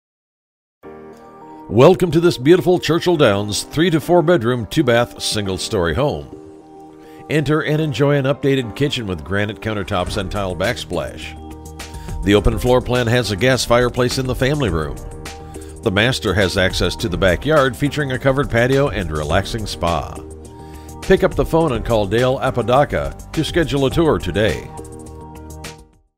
9254 Deddington Way Sacramento, CA | ColdwellBankerHomes.com
9254 Deddington Way, Sacramento, CA

Welcome to the beautiful Churchill Downs three to four bedroom, two bathroom, tile roof, single-story home. Enter inside and enjoy a beautiful updated kitchen with granite countertops and tile backsplash and updated bathrooms with granite countertops. The open floorplan has a cozy gas fireplace in t